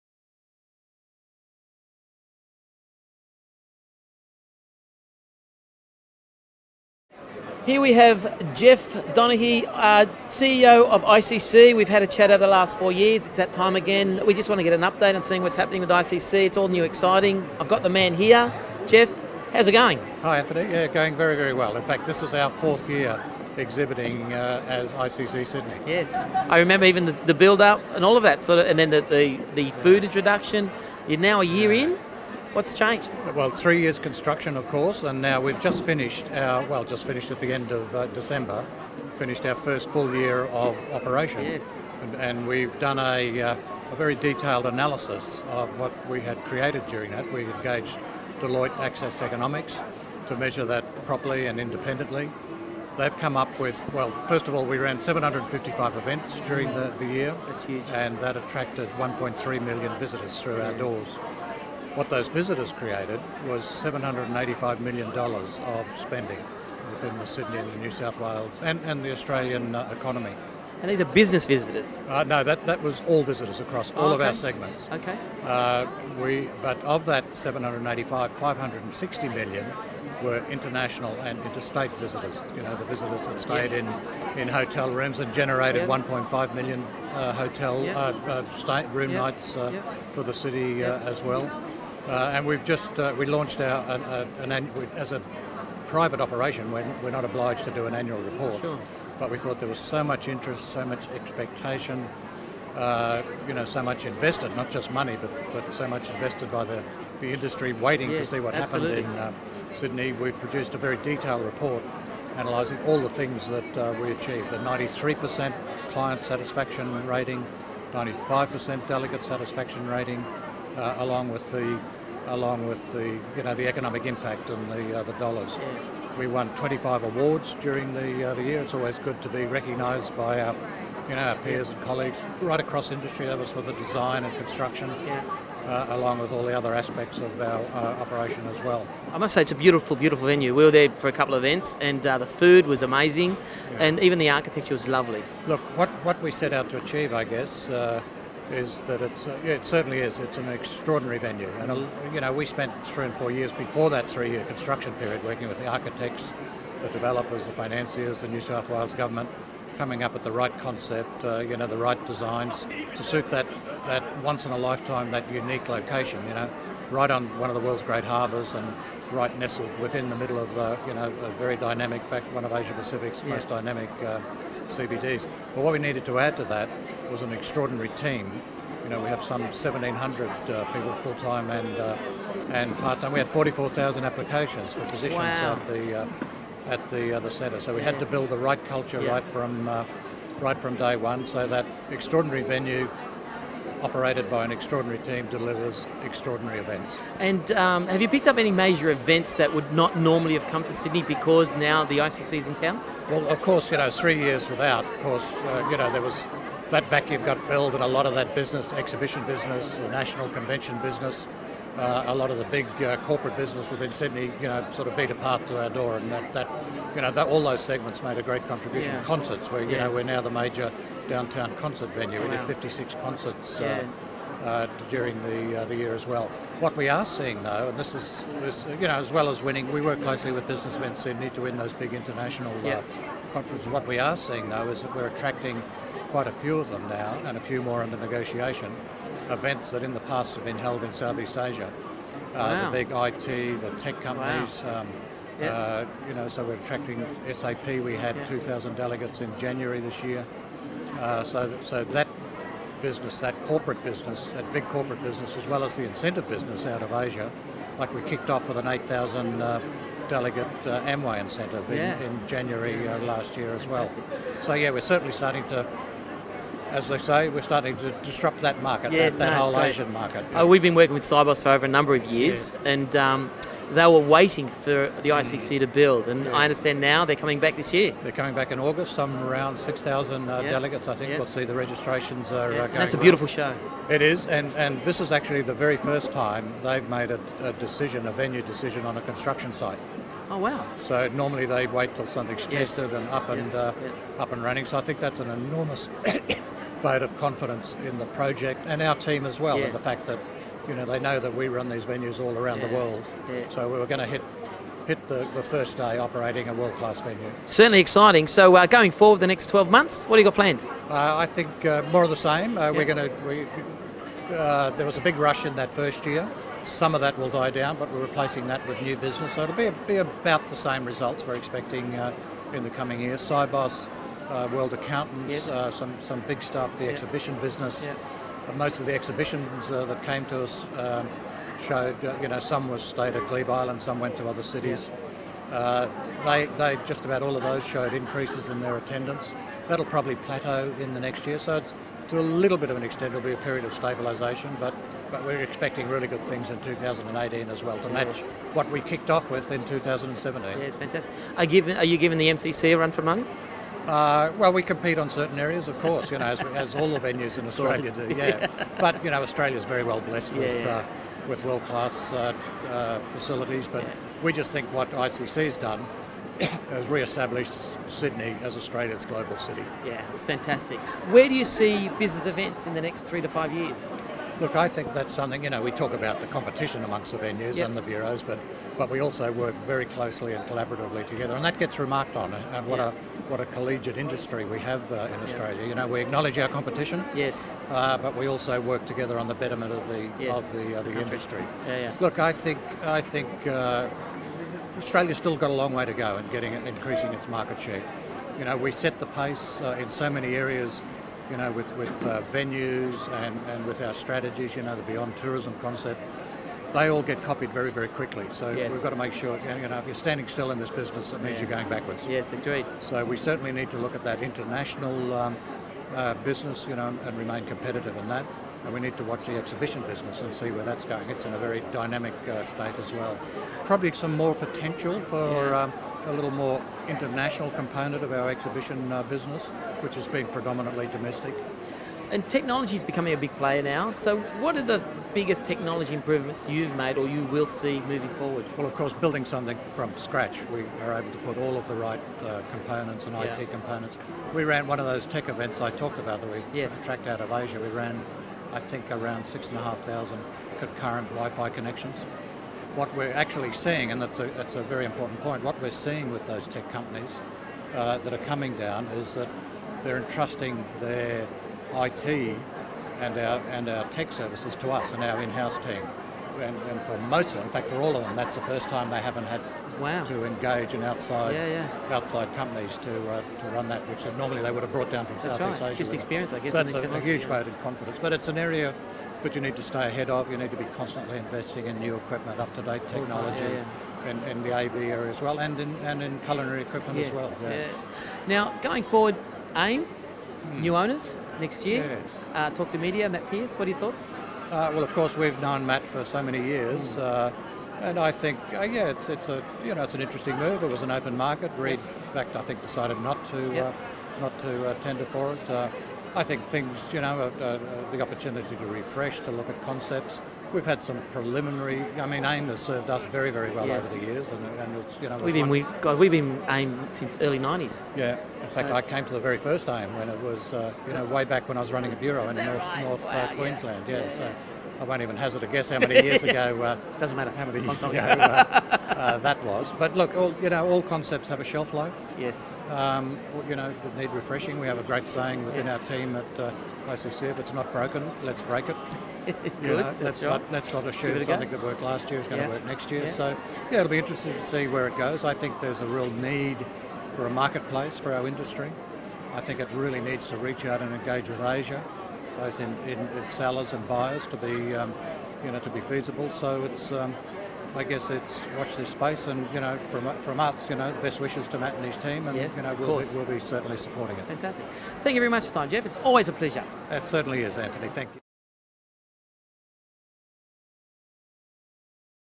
Geoff Donaghy - ICC Sydney
- Anthony speaks to Geoff Donaghy to find out the latest news from the ICC Sydney.